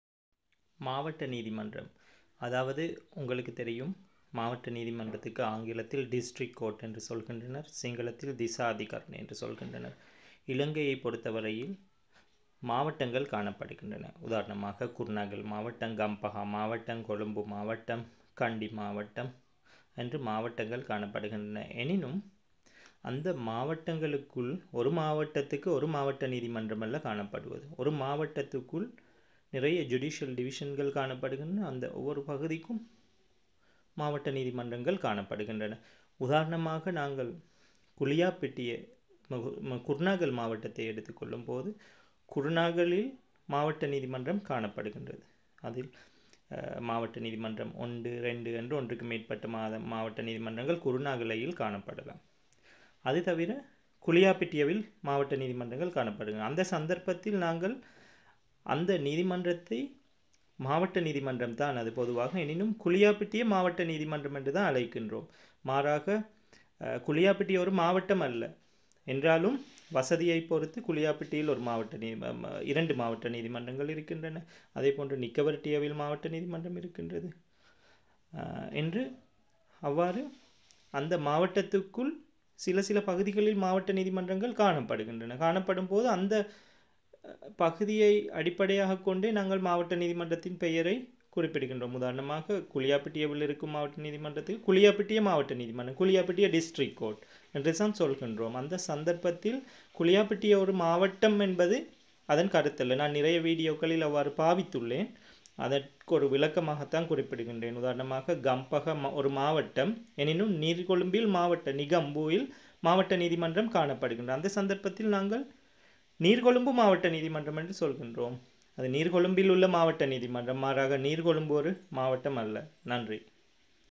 District Court // மாவட்ட நீதிமன்றம் // Sri Lanka // Tamil தமிழ் // MYMA Legal Awareness
Court System of Sri Lanka
இலங்கையின் நீதிமன்றங்கள் தொடர்பான அறிவினைப் பெறுவதற்கு கீழ் உள்ள லிங்க் ஐ கிளிக் செய்யவும்